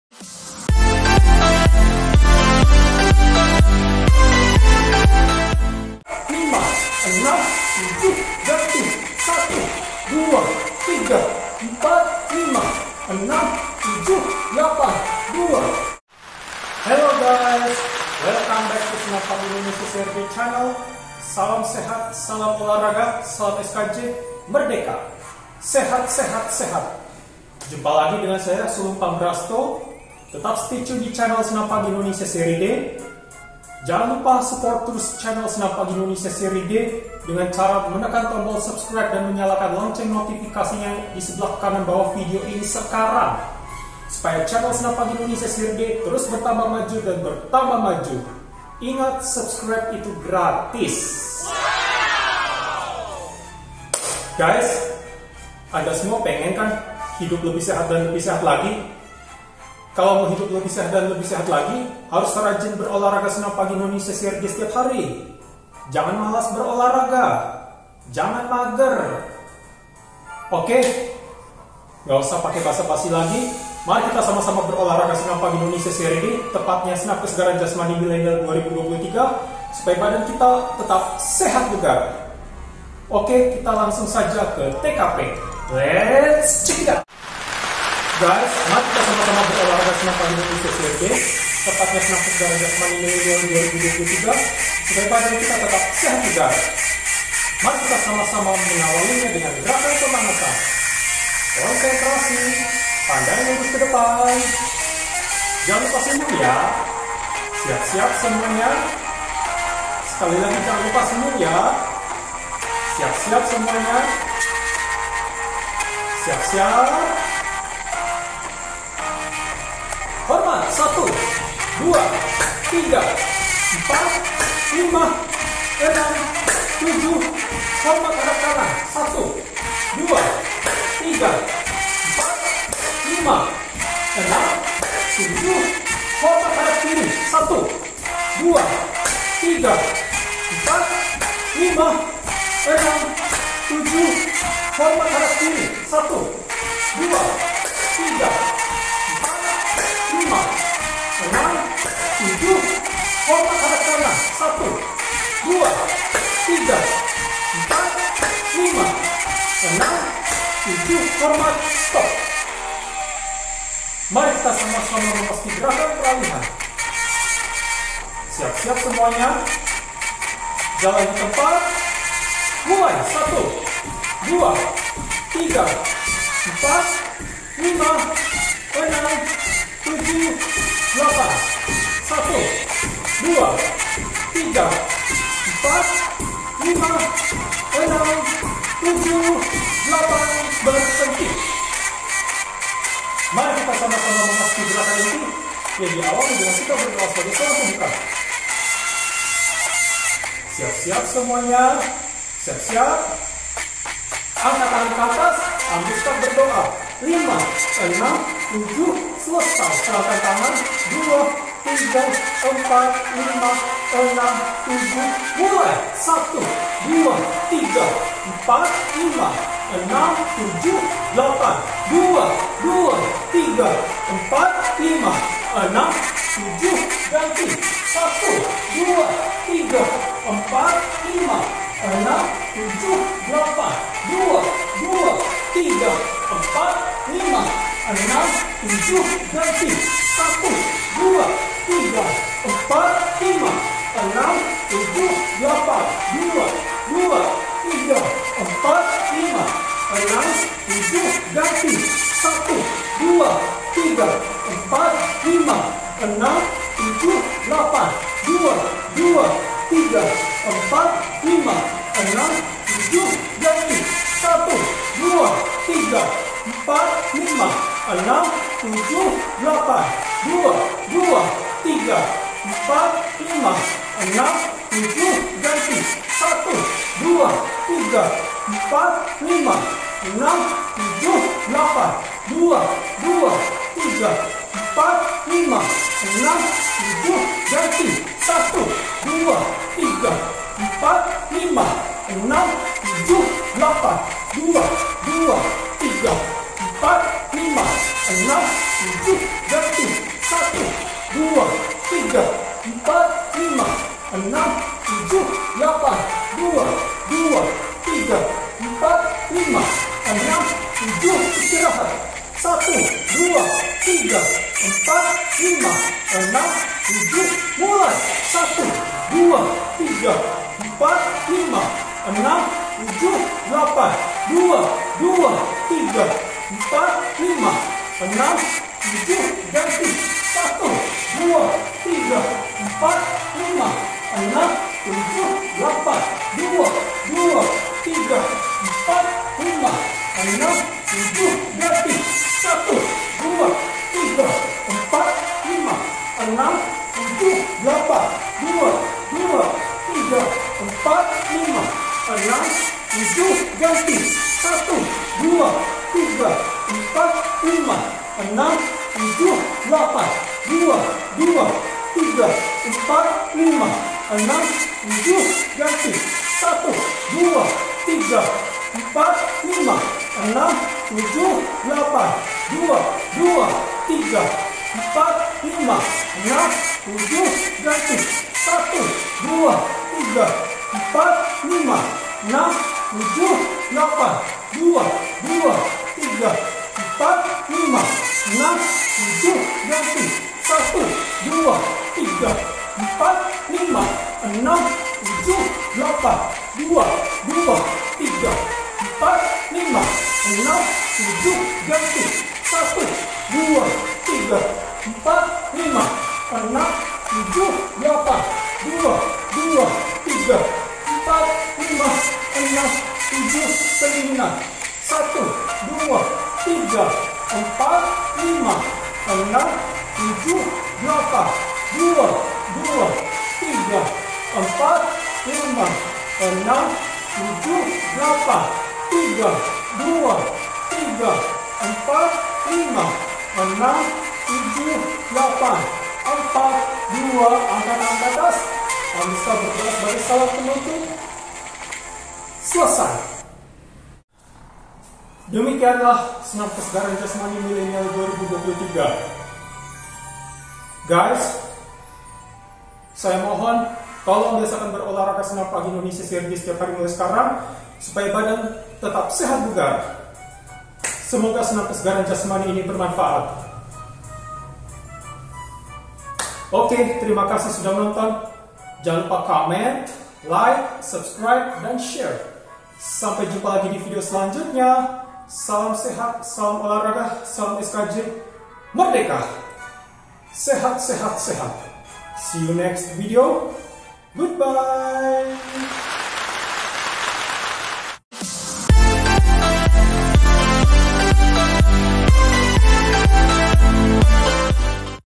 EPISODE 35 SKJ MILENIAL 2023 EDISI 2022 02/11/2022
Judul: EPISODE 35 SKJ MILENIAL 2023 EDISI 2022 02/11/2022

Salam SKJ! 💪
Mari kita biasakan berolahraga Senam Pagi Indonesia Seri D, tepatnya Senam Kesegaran Jasmani Milenial 2023 setiap hari mulai sekarang, supaya badan kita tetap sehat bugar. Semoga bermanfaat...! 😀🙏

setelah selesai menonton video ini sekarang.

Salam SKJ, 💪
SULUNG PAMBRASTO